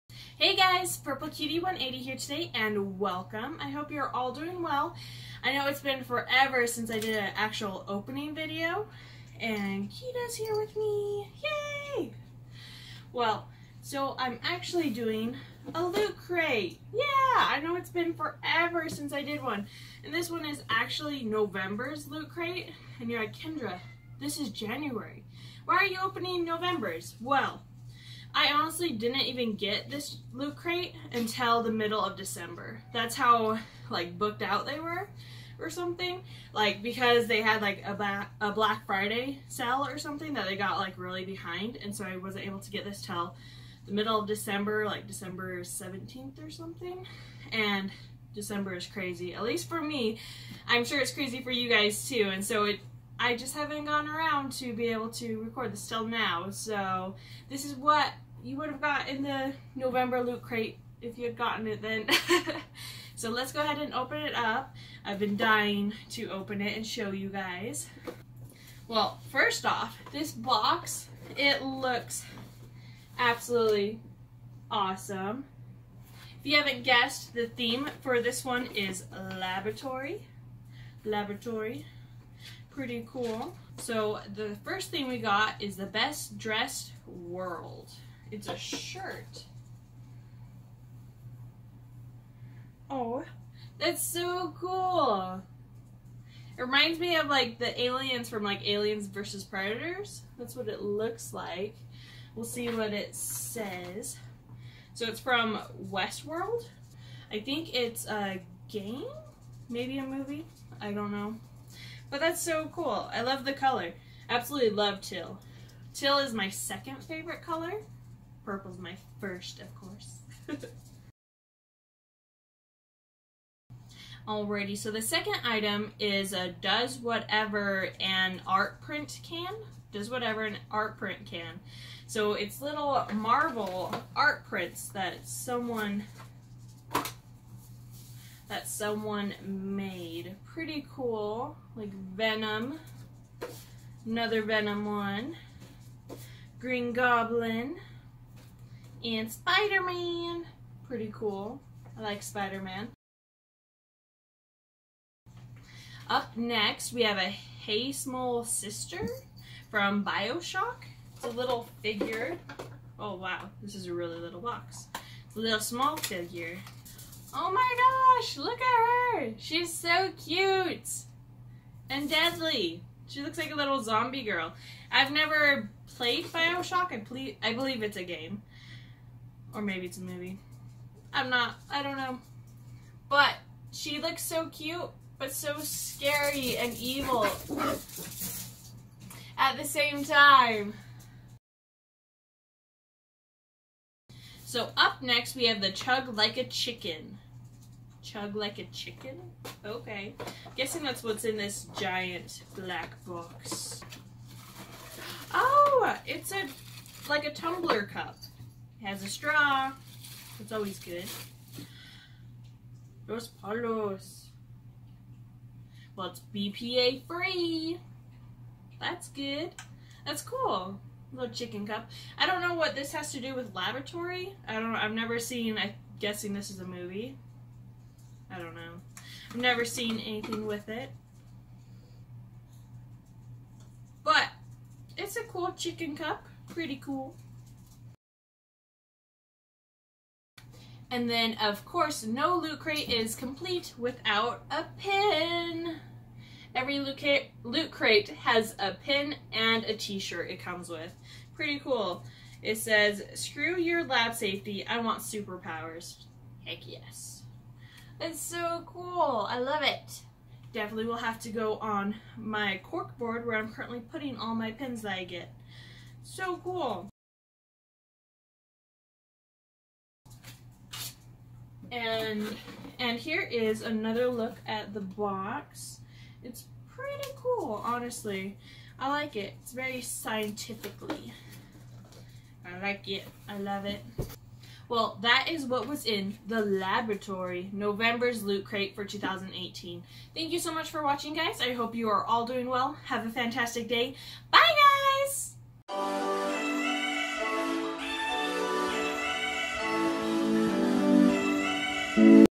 Loot Crate November 2018
In this opening video we are opening a Loot Crate from November, yes its January and November was like 3 months ago, but I didn't get this Loot Crate tell the middle of December and then I've been buy :P But here it is now :)